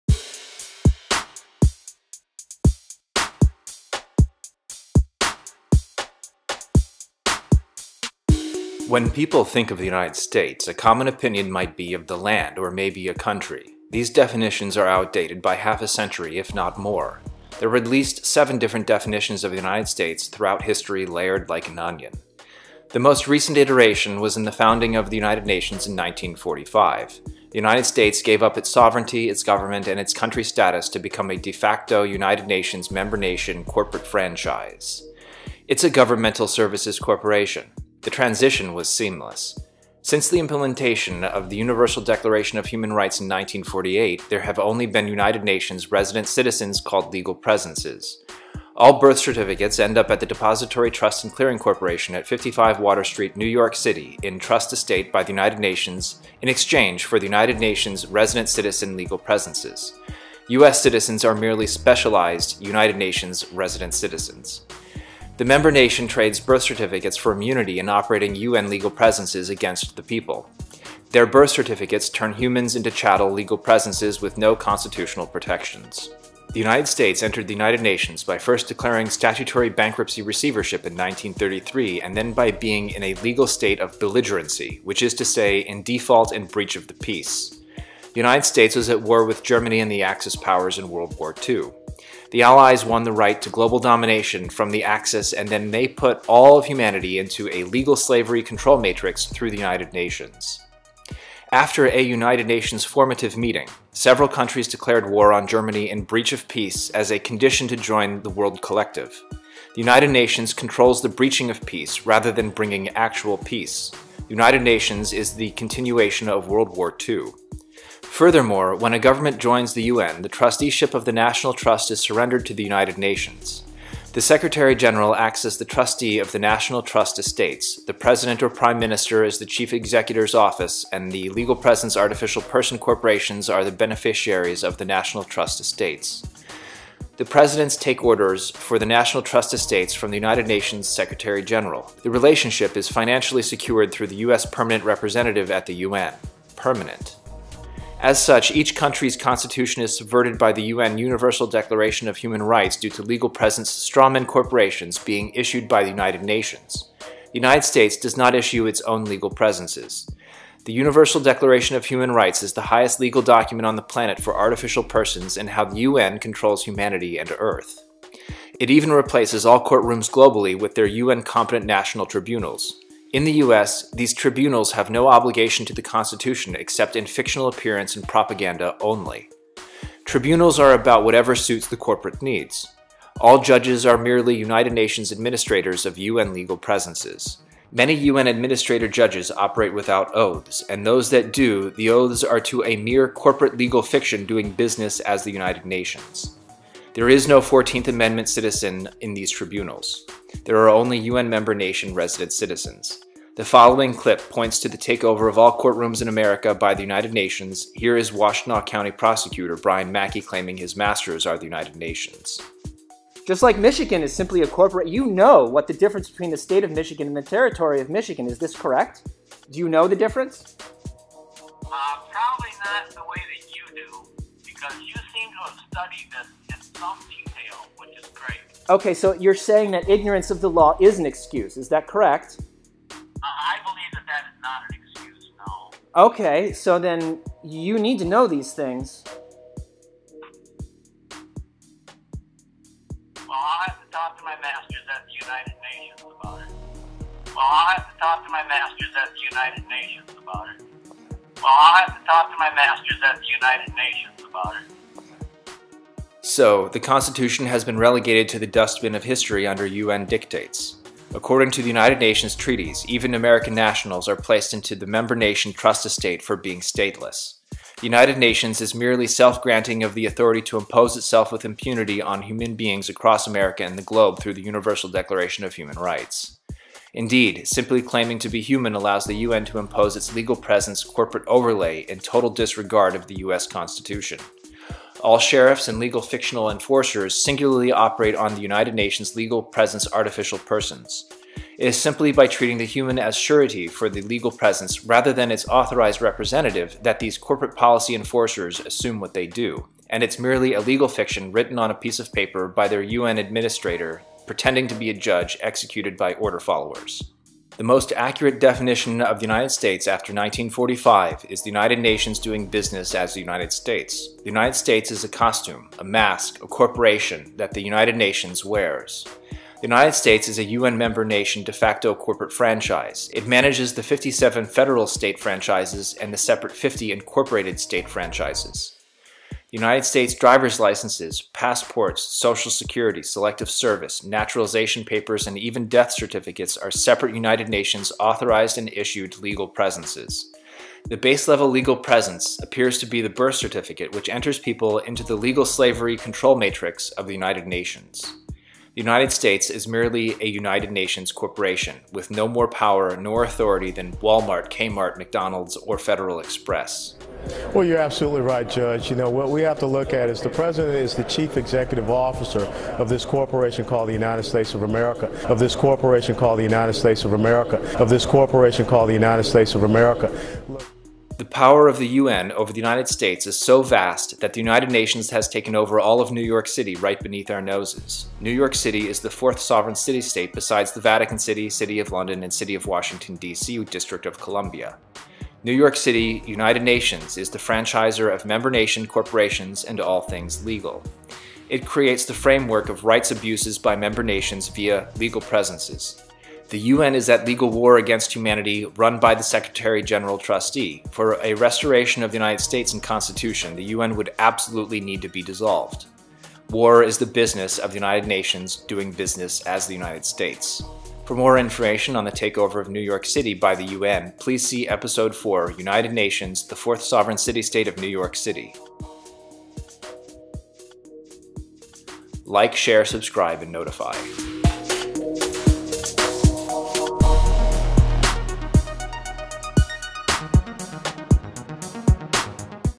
💥e9- What is the United States? 🇺🇸🎭 A United Nations Member-Nation Corporation 🇺🇳
Shocking revelations about the true legal status of the United States since 1945. Did you know the US government and sovereignty was quietly ceded to become a UN franchise corporation?

Learn how the founding of the UN in 1945 was used to covertly convert nations like the US into member-state corporations under total UN control.

Without most realizing it, UN declarations and treaties have replaced the Constitution as supreme law. Humans are unlawfully trafficked as property using birth certificates. All courtrooms globally were franchised with new owners into UN competent national tribunals under the UN UDHR.

The American institutions we took for granted - our rights, elections, presidents - are theatrical props for maintaining confusion while the UN runs the US corporation franchise we falsely call a "Nation."

This mind-blowing video will uncover why American freedoms are vanishing - we have been legally enslaved by creative paperwork. We must wake up to how the USA died in 1945 as a sovereign nation and learn the truth about this United Nations-run Law of War occupying belligerent power falsely posing as our government.

Chapters
0:00 Intro
0:08 7 Different United States - The Current UN Version
0:39 UN UDHR Nationals - Birth Certificates - Chattel
1:16 UN control matrix from WW2, US surrenders
1:57 UN is Trustee, President Executor of "People in Trust"
2:27 Constitution is Subverted by UN UDHR via UN Nationals
2:40 📜 UN UDHR & Article 8 - UN has Taken Over all "Courtrooms" globally
3:32 📞 Washtenaw County Head Prosecutor Brian Mackie P-25745
4:03 📞 Mackie P-25745 "Masters" are the UN
4:16 No Constitution - We're all Human Stateless, so the UN UDHR
5:07 What is the United States under the UN? & Birth Certificates
5:57 📺 Rep. Allen West FL-R "US is a Corporation"
6:10 UN taken over NYC, 4th Sovereign City-State
7:04 Outro

The UN Universal Declaration of Human Rights - The Definitive document of human slavery: